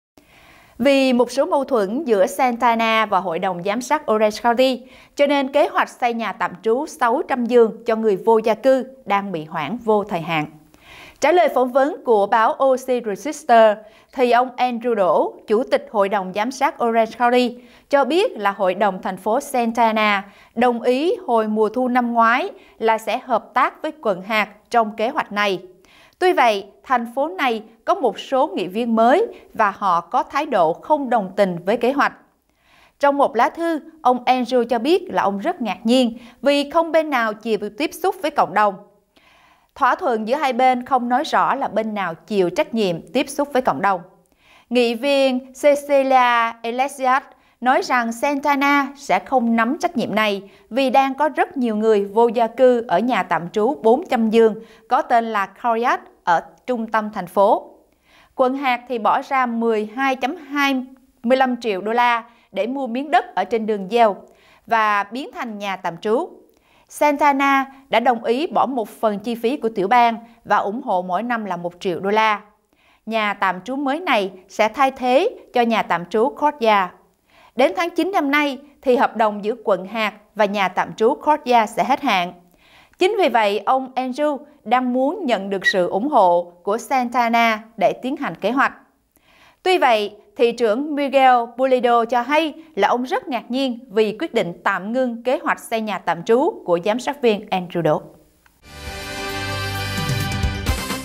Kế hoạch nhà tạm trú 600 giường của Orange County bị hoãn vô thời hạn
Vì một số mâu thuẫn giữa Santa Ana và Hội Đồng Giám Sát Orange County, kế hoạch xây nhà tạm trú 600 giường cho người vô gia cư đang bị hoãn vô thời hạn. Trả lời phỏng vấn của OC Register, ông Andrew Đỗ, chủ tịch hội đồng Giám sát Orange County cho biết Hội Đồng Thành Phố Santa Ana đồng ý hồi mùa Thu năm ngoái sẽ hợp tác với quận hạt trong kế hoạch này.